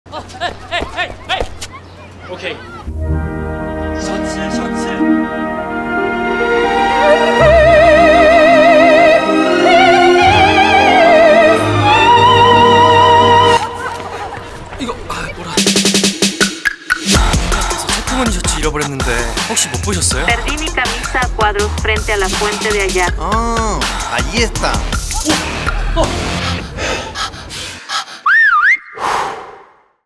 Galaxy S24 Ultra Official Film: Interpreter | Samsung
Translate on the spot, just like that. Now, you have a personal interpreter, the Galaxy S24 Ultra, in your hand 🔊